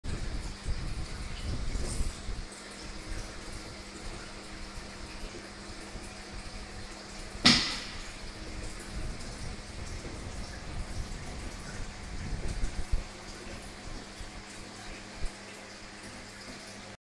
Tiger shovelnose catfish display incredile brust of speed when feeding
2 x 9"  Tiger shovelnose catfish & 3 x 9" Spotted Gar fish feeds at lightning speed.